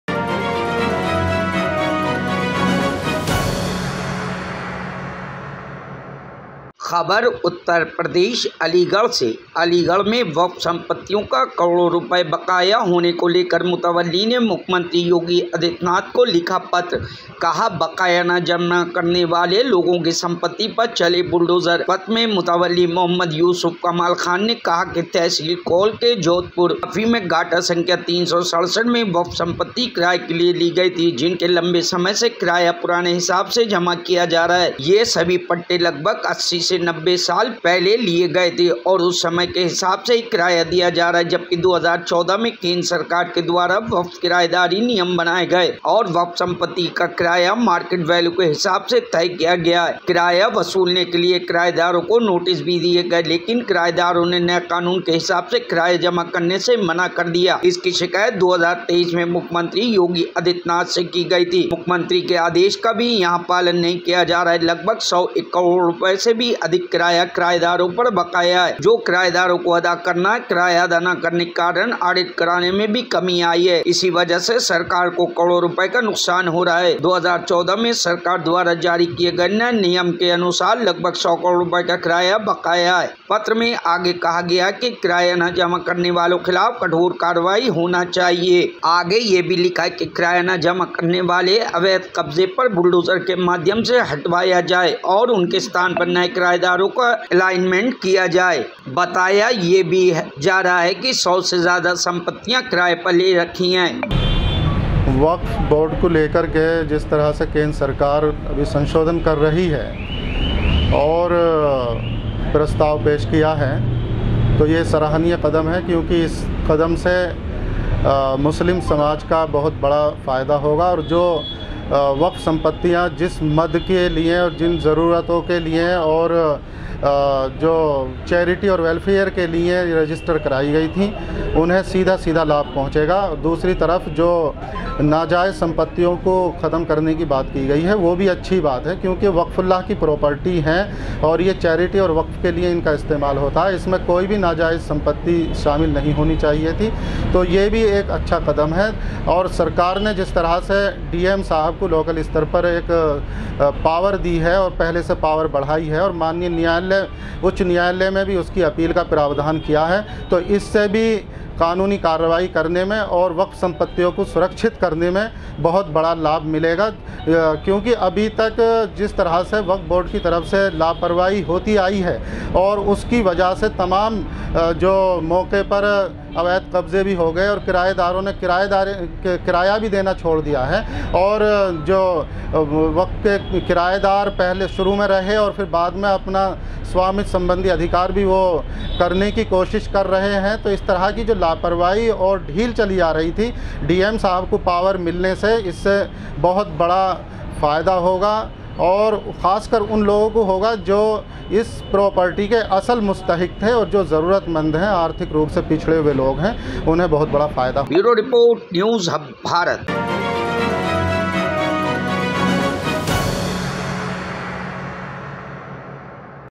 Aligarh : क्फ संपत्तियों का किराया न देने वाले लोगों पर चलाया जाए बुलडोजर | NHB
अलीगढ़ में वक्फ संपत्तियों का करोड़ों रुपए बकाया होने को लेकर मुतवल्ली ने मुख्यमंत्री योगी आदित्यनाथ को लिखा पत्र, कहां-बकाया न जमा करने वाले लोगों की संपत्ति पर चले बुलडोजर, पत्र में मुतवल्ली मोहम्मद यूसुफ कमाल खान ने कहा है कि तहसील कोल के जोधपुर माफी में गाटा संख्या 367 मैं वक्फ संपत्ति किराए के लिए ली गई थी, जिनके लंबे समय से किराया पुराने हिसाब से जमा किया जा रहा है, यह सभी पट्टे लगभग 80 से 90 साल पहले लिए गए थे, और उस समय के हिसाब से ही किराया दिया जा रहा है, जबकि 2014 में केंद्र सरकार के द्वारा वक्फ किराएदारी नियम बनाए गए और वक्फ संपत्ति का किराया मार्केट वैल्यू के हिसाब से तय किया गया है, किराया वसुनने के लिए किराएदारों को नोटिस भी दिए गए हैं, लेकिन किरायेदारों ने नए कानून के हिसाब से किराया जमा करने से मना कर दिया है, इसकी शिकायत 2023 में मुख्यमंत्री योगी आदित्यनाथ से की गई थी, मुख्यमंत्री के आदेश का भी यहां पालन नहीं किया जा रहा है, लगभग 100 करोड रुपए से भी अधिक किराया किराएदारों पर बकाया है, जो किराएदारो को अदा करना है, किराया अदा न करने के कारण ऑडिट करने में भी कमी आई है, इसी वजह से सरकार को करोड़ों रुपए का नुकसान हो रहा है, 2014 में सरकार द्वारा जारी किए गए नए नियमों के अनुसार लगभग 100 करोड रुपए का किराया बकाया है,पत्र में आगे कहा है कि किराया न जमा करने वालों के खिलाफ कठोर कार्रवाई होनी चाहिए, आगे यह भी लिखा है कि किराया न जमा करने वाले अवैध कब्जे पर बुलडोजर के माध्यम से हटवाया जाए और उनके स्थान पर नए किराएदारों का एलाइनमेंट किया जाए, बताएं यह जा रहा है कि 100 से ज्यादा संपत्तियां किराए पर ले रखी है।